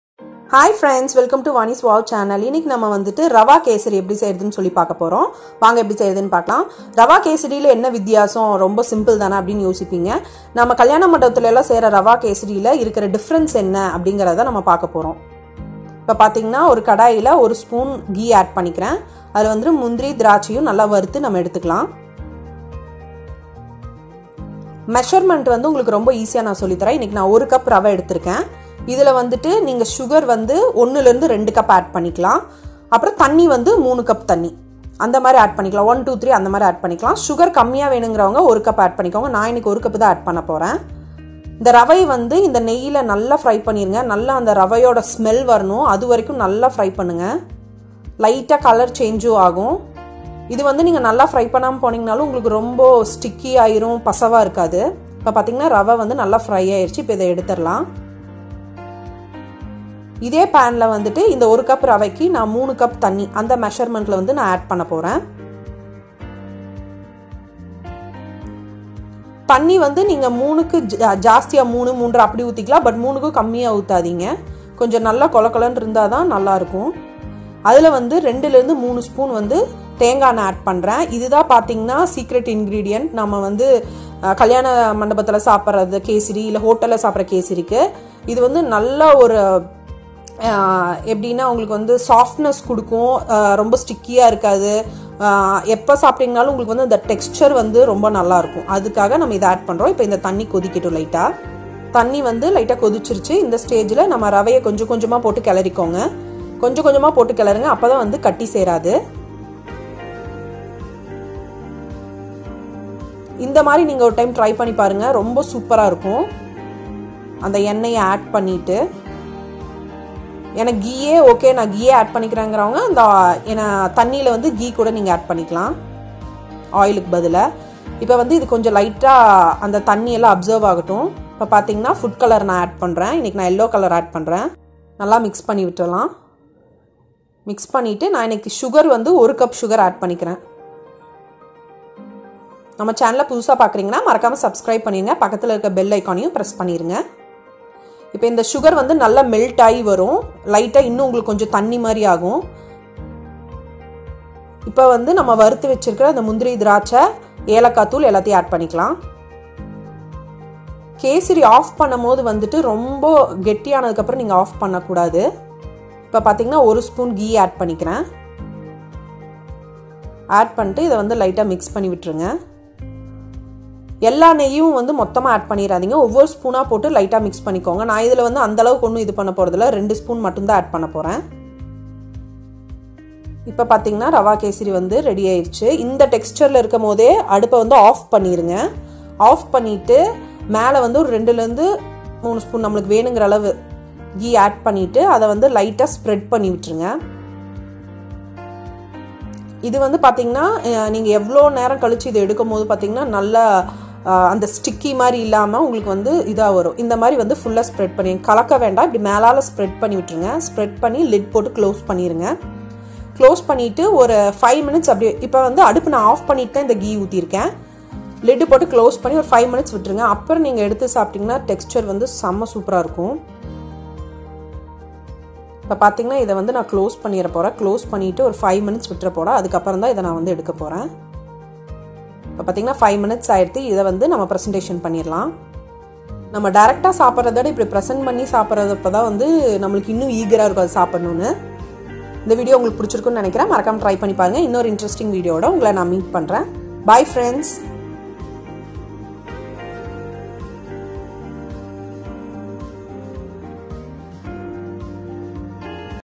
Rava Kesari Recipe in Tamil#Kalyana Veedu Rava Kesari#Sweet Recipe#how to make rava kesari in tamil
Hi fiends,
In this video I shared Rava Kesari in the style of function mode.

Vani's Woww